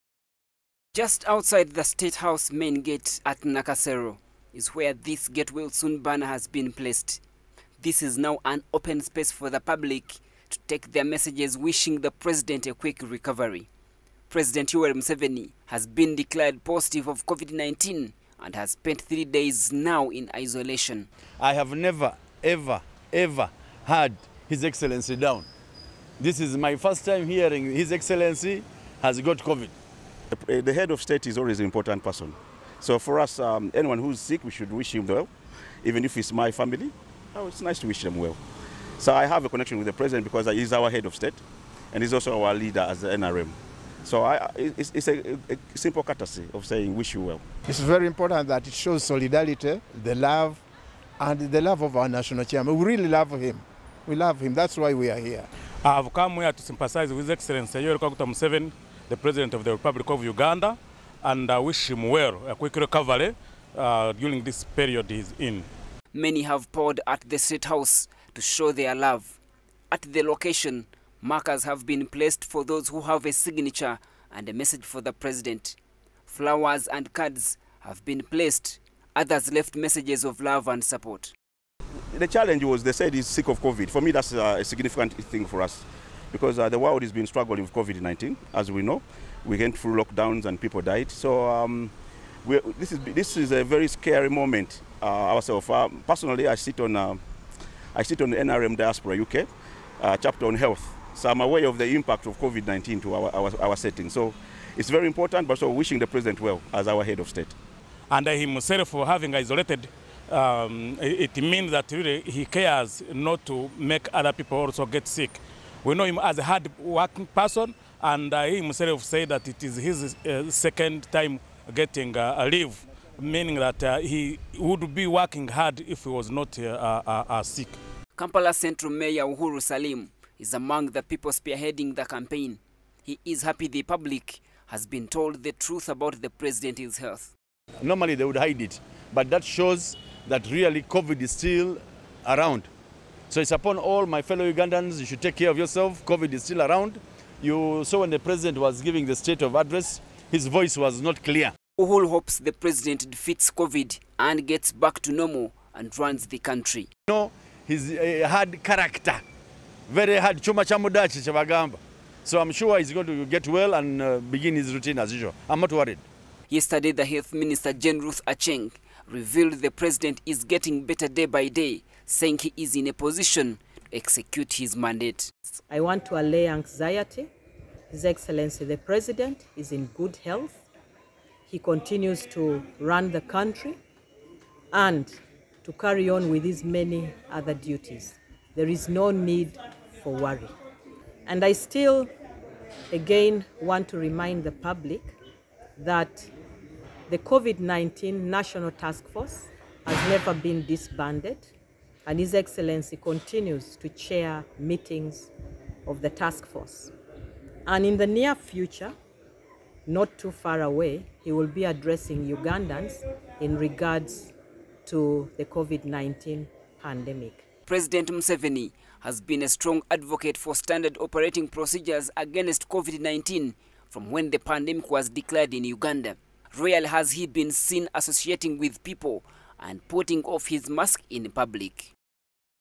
State House Open To Get Well Messages For President Museveni
Many NRM diehards and other well-wishers have expressed their love and support towards President Museveni wishing him a quick recovery.
Many of these have participated in the get well campaign where an open public space has been opened at state house Nakasero for members of the public to deliver their messages.
The members we spoke to say they are confident the president will soon get back at his feet and run the country.